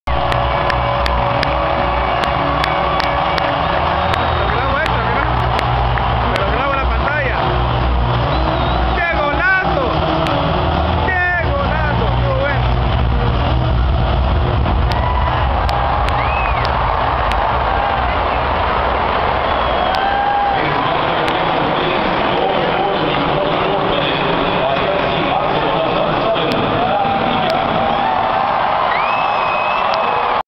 David Villa - Gol de cabeza, in the Barca vs America game 2011
David Villa Scores a header to make it 1-0 in the 2-0 final of the Barca vs Amwrica game Agust 6 2011